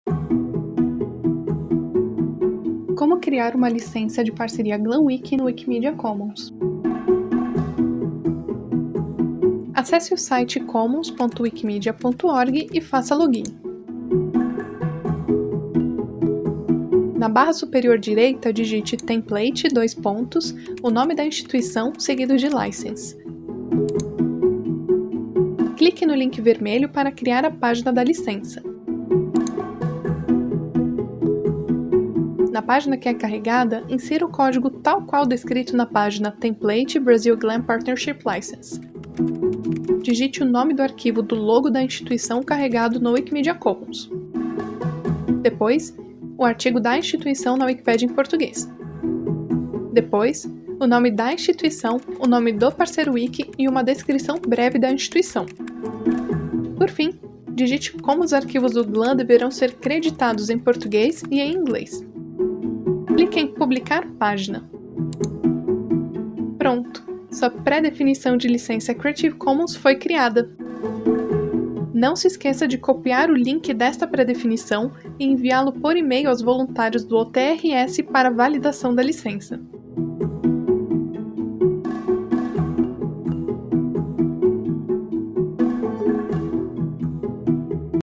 Como criar uma licença de parceria GLAM Wiki no Wikimedia Commons
Vídeo tutorial sobre como criar uma licença de parceria GLAM Wiki no Wikimedia Commons.

Autoria: Wiki Movimento Brasil
Roteiro/Ilustração/Animação: Éder Porto
Narração: Érica Azzellini